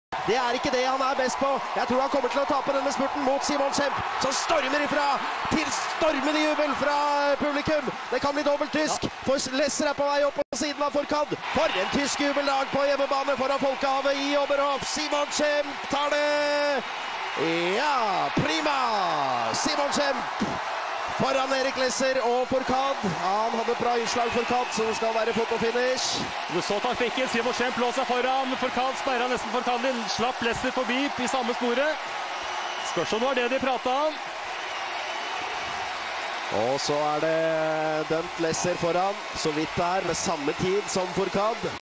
(Mass start - Oberhof 2017) SUPER FINISH Simon Schempp, Martin Fourcade & Erik Lesser
SUPER FINISH - Simon Schempp, Martin Fourcade & Erik Lesser - mass start - Oberhof 2017